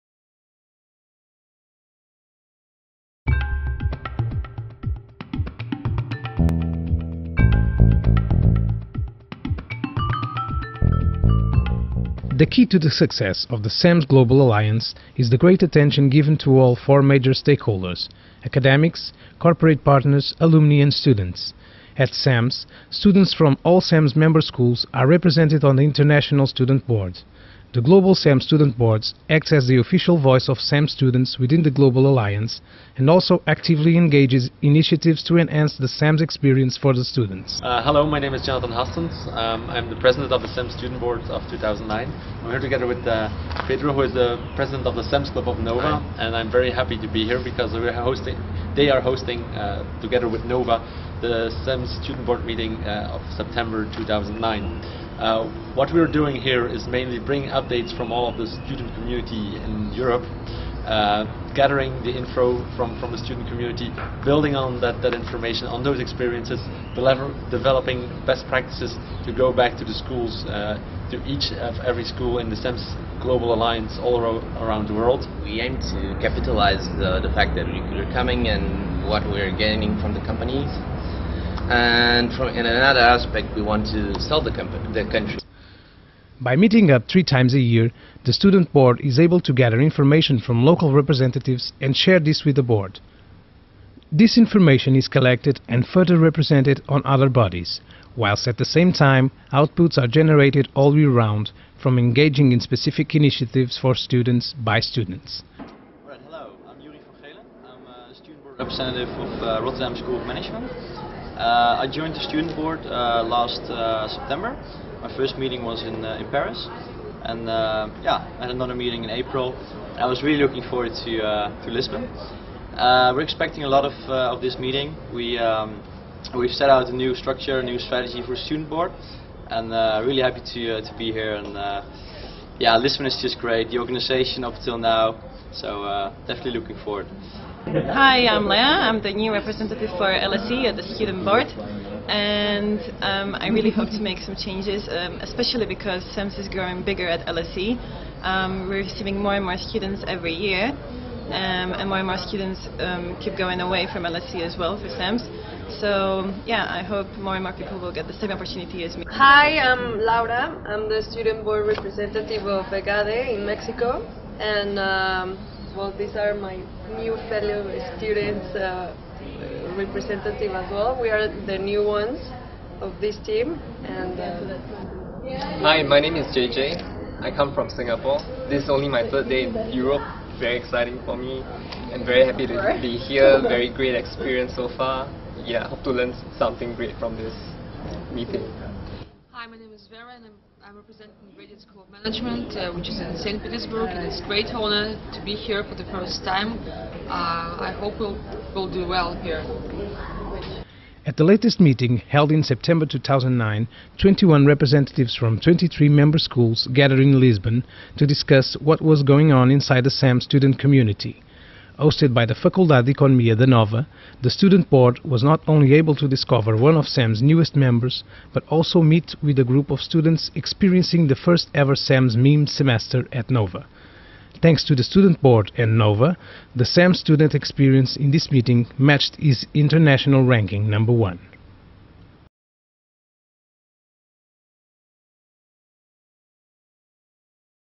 CEMS International Student Board - September '09
By meeting up to three times a year, the Student Board is able to gather information from local representatives and share this with the Board. This information is collected and further represented on other bodies, whilst at the same time outputs are generated all year round, from engaging in specific initiatives for students, by students.

At the latest meeting held in September 2009, 21 representatives from 23 member schools gathered in Lisbon to discuss what was going on inside the CEMS student community.

Hosted by the Faculdade de Economia da NOVA, the Student Board was not only able to discover one of CEMSs newest members, but also meet with the group of students experiencing the first ever CEMS MIM semester at NOVA